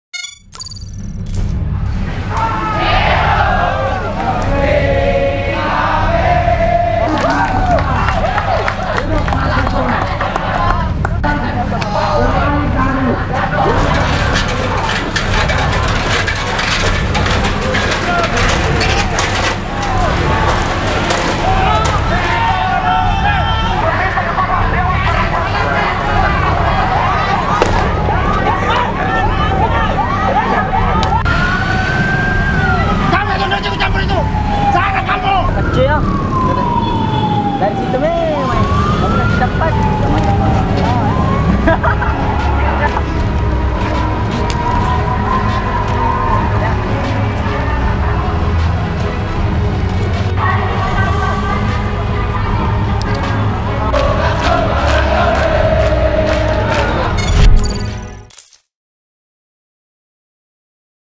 Lagi, Pengunjuk Rasa dan Polisi Bentrok di Makassar
Unjuk rasa mahasiswa dan pelajar di kota Makassar berlangsung ricuh. Kericuhan bermula saat mahasiswa mamaksa masuk gedung DPRD Sulawesi Selatan yang dijaga aparat kepolisian.